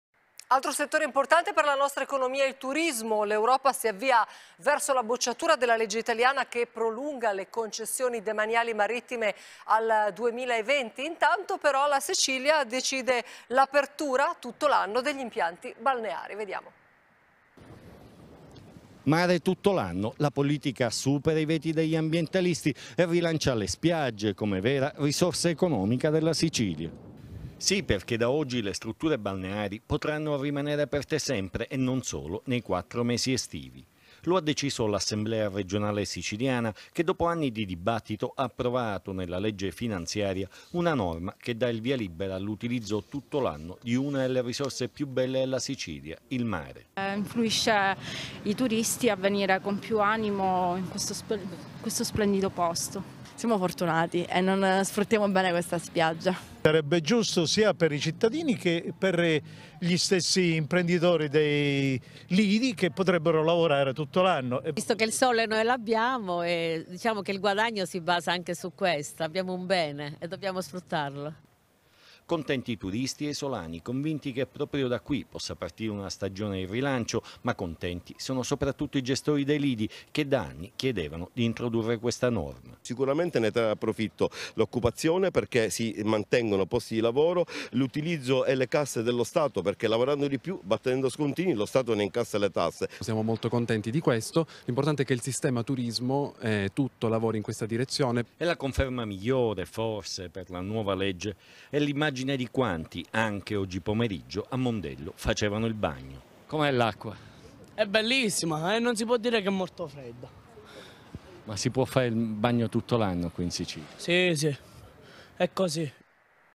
Mare tutto l'anno, la Regione dice sì
Per Mondello la possibilità di vivere pienamente 12 mesi, ma tutto questo nel rispetto dell'ambiente.
Beppe Castellucci: "È il momento di fare tutti la nostra parte, Mondello non deve essere ricordata solo d'estate. Ed ogni giorno, per 12 mesi, noi ci occupiamo di Mondello"

Seguici